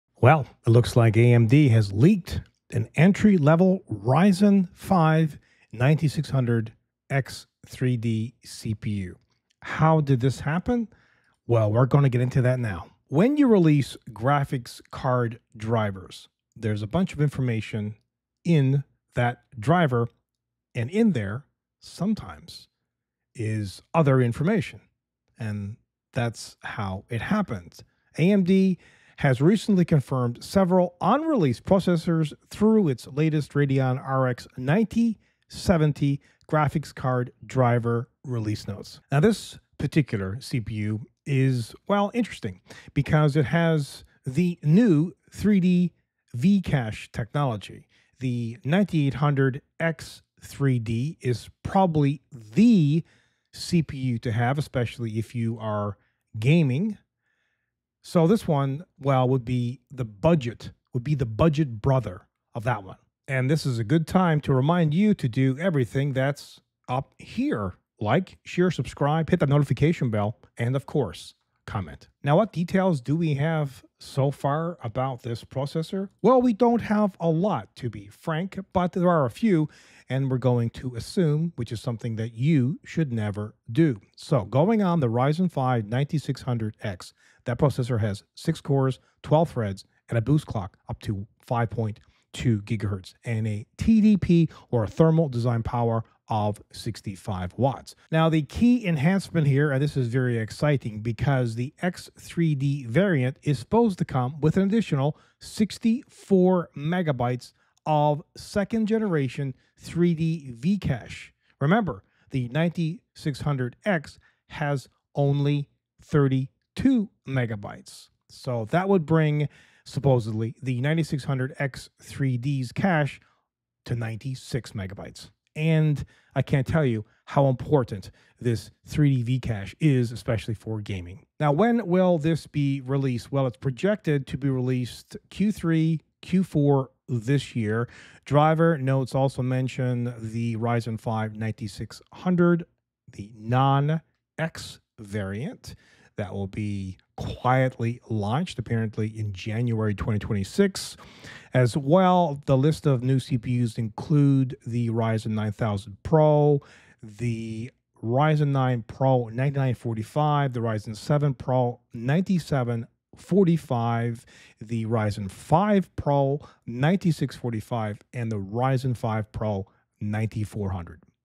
AMD Leaked New CPUs - 9600X3D & More
AMD has recently confirmed several unreleased processors through its latest Radeon RX 9070 graphics card driver release notes. Among these is a new entry-level desktop CPU, the Ryzen 5 9600X3D, which integrates AMD's gaming-optimized 3D V-Cache technology. This technology significantly boosts gaming performance by providing a larger L3 cache, enabling faster CPU access to game data, leading to higher frame rates and smoother gameplay. The introduction of the Ryzen 5 9600X3D expands AMD's existing X3D lineup and offers a new entry point for PC builders and gamers seeking the latest Zen 5 architecture on the AM5 platform combined with 3D V-cache.

While full specifications for the Ryzen 5 9600X3D have not been officially detailed, it is anticipated to largely mirror the Ryzen 5 9600X. This implies a configuration of six cores and 12 threads, with boost clock speeds reaching up to 5.2 GHz and a Thermal Design Power (TDP) of 65W. The key enhancement for the 9600X3D variant is expected to be an additional 64MB of second-generation 3D V-cache, bringing its total L3 cache to 96MB, an increase from the 9600X's standard 32MB.

According to tech leaker MelodicWarrior, the Ryzen 5 9600X3D will initially be available to system integrators, with a broader release for the DIY market projected between late Q3 and early Q4 2025. The driver notes also mention the Ryzen 5 9600, a non-X variant that was quietly launched earlier in January. Furthermore, the list includes four new CPUs under the Ryzen 9000 PRO series: the Ryzen 9 PRO 9945, Ryzen 7 PRO 9745, Ryzen 5 PRO 9645, and Ryzen 5 PRO 9400. Specific architectural details regarding whether these enterprise and workstation-focused CPUs are based on the Granite Ridge architecture, like other Ryzen 9000 series processors, remain unconfirmed.

Adding to the roster of unreleased processors, the Ryzen 7 9700F was recently identified in AGESA microcode update documentation. The "F" designation for this CPU typically indicates the absence of an integrated GPU (iGPU), mirroring Intel's naming conventions. This 9700F model is expected to feature an 8-core, 16-thread configuration, identical to the standard Ryzen 7 9700, but without the integrated Radeon graphics. This particular chip could be targeted at cost-conscious gamers and PC builders who intend to use a discrete graphics card, potentially allowing AMD to offer it at a more competitive price point.